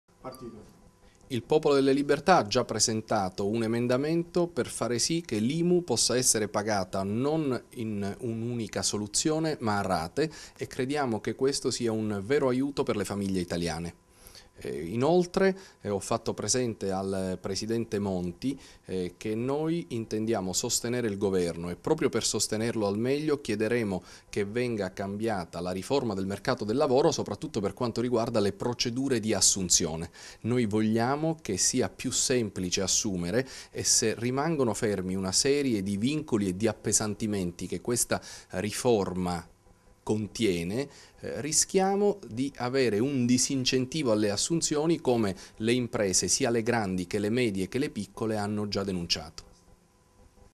ALFANO: IMU A RATE
Imu a rate. Questa la proposta che il Pdl ha presentato in parlamento per venire incontro alle difficoltà dei cittadini. Il lavoro è l'altro tema che Angelino Alfano, segretario del Pdl, proporrà al presidente del Consiglio Mario Monti.